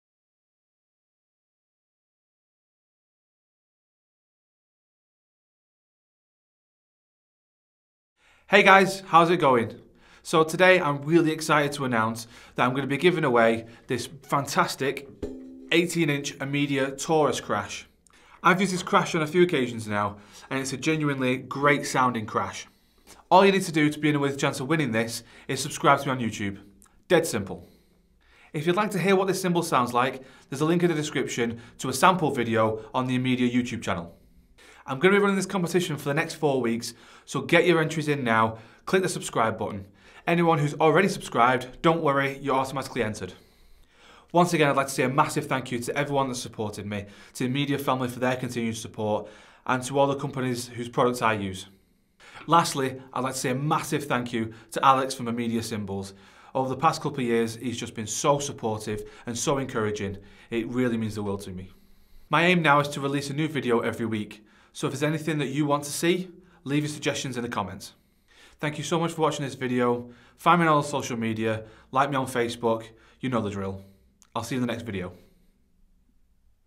Amedia 18" Taurus Crash - Cymbal Giveaway - OwenAlec Drumming
Having been a part of the Amedia Family for over 2 years now, I still get overwhelmed with the quality of the products and the continued support from everyone! So I'd like to pass on the love, by giving away this 18" Amedia Taurus Crash.

Proudly Endorses Amedia Cymbals.